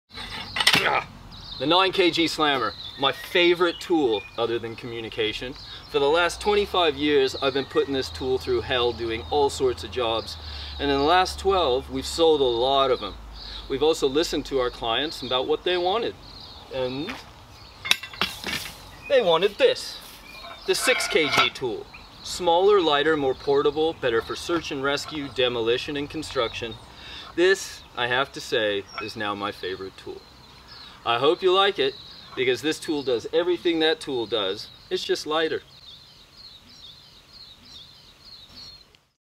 Introducing the 6kg Slammer Tool
A message from The Slammer Tool founder TJ on why the 6 kg Slammer Tool has become his go-to tool for all jobs!

It's built from the same sturdy materials as the 9kg Slammer and does all the same jobs, it's just lighter and easier to work with!
Purchase Your Own Slammer Tool!
Find Us Online!
Slammer Tool - Simple Made Strong